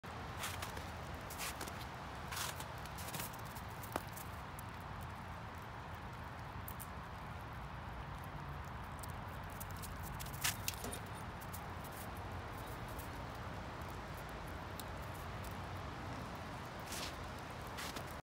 Day 186 with Quirrel the squirrel. Quirrel doesn't like almonds. Maybe it is too hard?
Day 186 with Quirrel the squirrel. Quirrel doesn't like almonds. Maybe it is too hard? There is no video in the past 23 days. Sometimes, I wasn't home when Quirrel was back and other people in the house just feed her and didn't take videos.I think she was back a few times within the past 23 days.